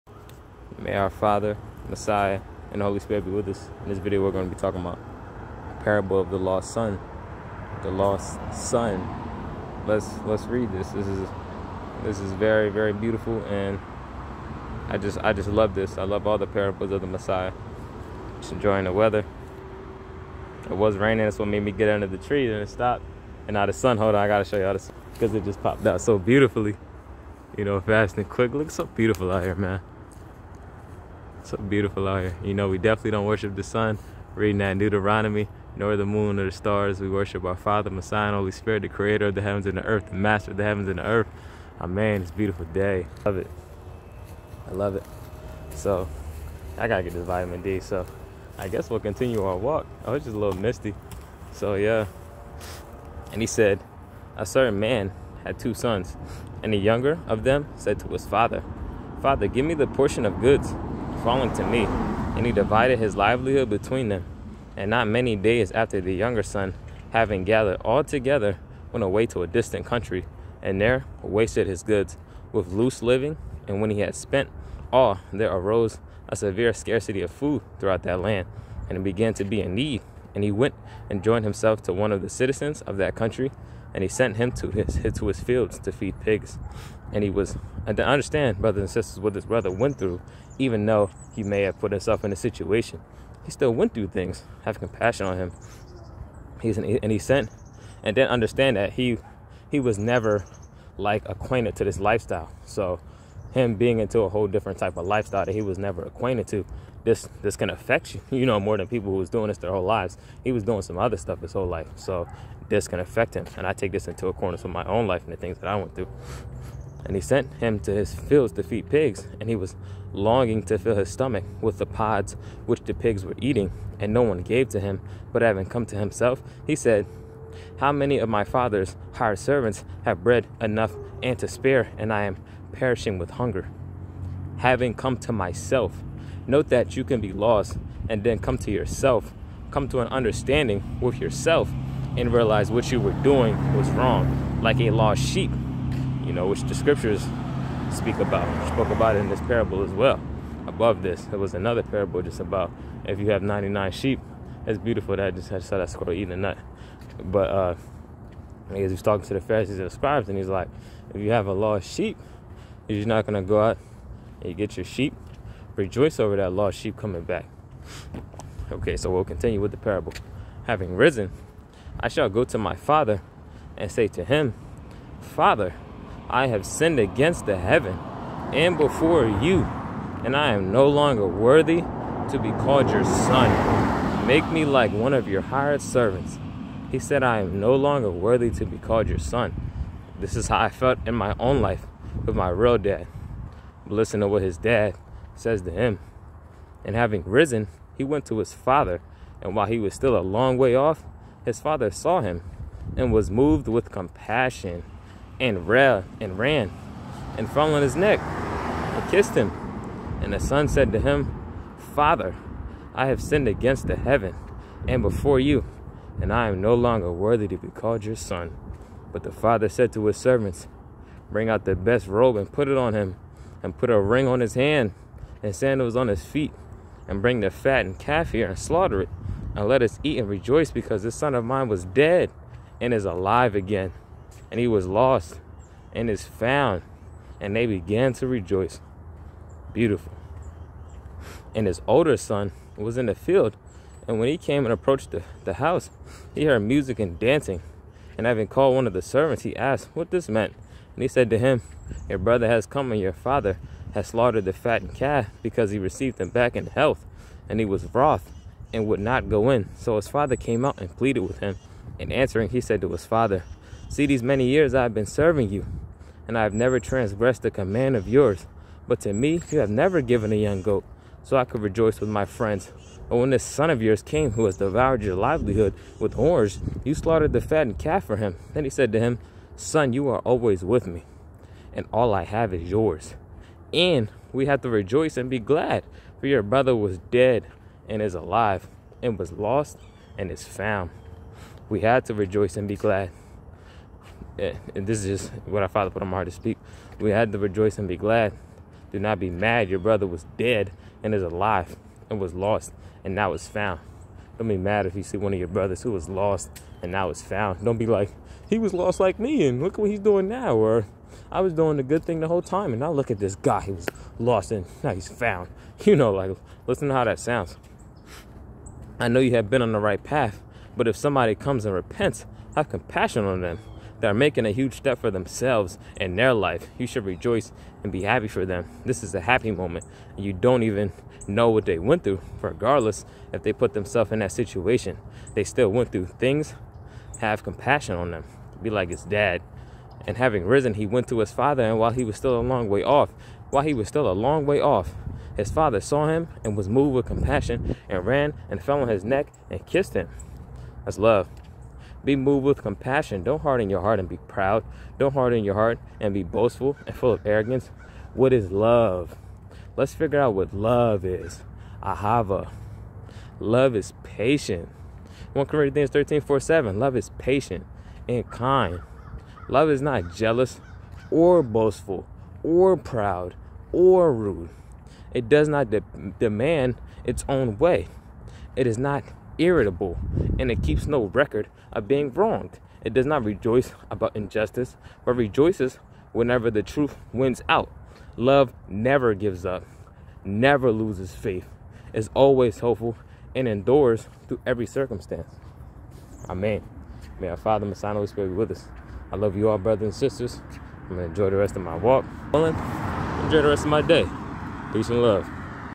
Parable of the lost son, What Is Love?
And He said, “A certain man had two sons, and the younger of them said to his father, ‘Father, give me the portion of goods falling to me.’ And he divided his livelihood between them. And not many days after, the younger son, having gathered all together, went away to a distant country, and there wasted his goods with loose living. “And when he had spent all, there arose a severe scarcity of food throughout that land, and he began to be in need. “And he went and joined himself to one of the citizens of that country, and he sent him to his fields to feed pigs. “And he was longing to fill his stomach with the pods which the pigs were eating, and no one gave to him. But having come to himself, he said, ‘How many of my father’s hired servants have bread enough and to spare, and I am perishing with hunger!”
Luqas (Luke) 15:11‭-‬17 TS2009

HAVING COME TO MYSELF - NOTE THAT
You can be lost and than come to yourself come to an understanding with yourself and realize what you were doing was wrong like a lost sheep which we will get to

Having risen, I shall go to my father and say to him, “Father, I have sinned against the heaven, and before you, and I am no longer worthy to be called your son. Make me like one of your hired servants.” ’
Luqas (Luke) 15:18‭-‬19 TS2009

He said I am no longer worthy to be called your son this is how I felt in my own life with my real dad but listen to what his dad says to him

“And having risen, he went to his father. And while he was still a long way off, his father saw him and was moved with compassion, and ran and fell on his neck and kissed him. And the son said to him, ‘Father, I have sinned against the heaven, and before you, and I am no longer worthy to be called your son.’ ” “But the father said to his servants, ‘Bring out the best robe and put it on him, and put a ring on his hand and sandals on his feet. And bring the fattened calf here and slaughter it, and let us eat and rejoice, because this son of mine was dead and is alive again, and he was lost and is found.’ And they began to rejoice.
Luqas (Luke) 15:20‭-‬24 TS2009

Beautiful
“And his older son was in the field, and when he came and approached the house, he heard music and dancing. “And having called one of the servants he asked what this meant. And he said to him, ‘Your brother has come, and your father has slaughtered the fattened calf because he received him back in health.’ ” “And he was wroth and would not go in. So his father came out and pleaded with him. “And answering, he said to his father, ‘See, these many years I have been serving you, and I have never transgressed a command of yours, but to me you have never given a young goat, so I could rejoice with my friends. But when this son of yours came, who has devoured your livelihood with whores, you slaughtered the fattened calf for him.’ “Then he said to him, ‘Son, you are always with me, and all I have is yours. And we had to rejoice and be glad, for your brother was dead and is alive, and was lost and is found.’ ”
Luqas (Luke) 15:25‭-‬32 TS2009

We had to rejoice and be glad do not be mad your brother was DEAD and is alive, and was lost and now is FOUND!

I know you have been on the right path but if somebody comes and repents have compassion on them they are making a huge step for themselves in there life you should rejoice and be happy for them this is a happy moment and you don't even know what they went through REGARDLESS IF THEY PUT THEMSELF IN THAT SITUATION THEY STILL WENT THROUGH THINGS HAVE COMPASSION ON THEM
BE LIKE HIS DAD
“And having risen, he went to his father. And while he was still a long way off, his father saw him and was moved with compassion, and ran and fell on his neck and kissed him.
Luqas (Luke) 15:20 TS2009

Be moved with compassion don't harden your heart and be proud don't harden your heart and be boastful and full of arrogance
Love is patient and kind. Love is not jealous or boastful or proud or rude. It does not demand its own way. It is not irritable, and it keeps no record of being wronged. It does not rejoice about injustice but rejoices whenever the truth wins out. Love never gives up, never loses faith, is always hopeful, and endures through every circumstance.
1 Corinthians 13:4‭-‬7 NLT